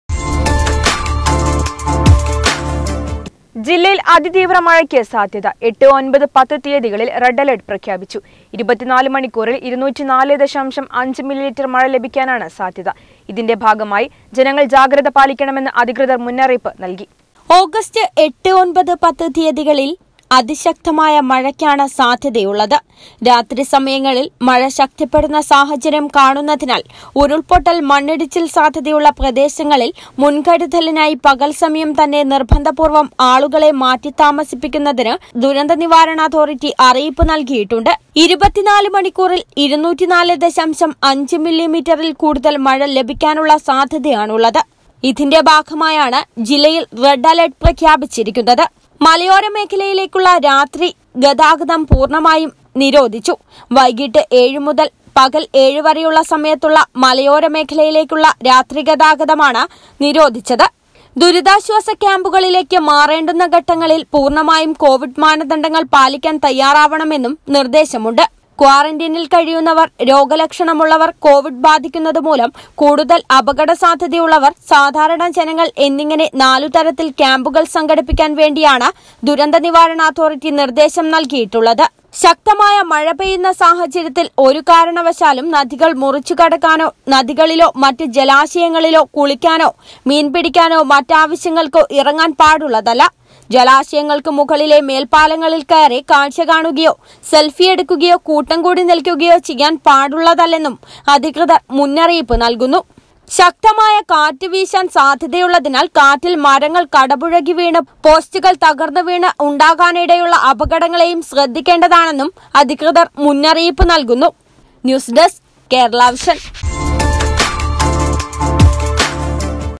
ജില്ലയിൽ അതി ശക്‌തമായ മഴ.
മലപ്പുറം ജില്ലയിൽ  റെഡ്  അലെർട്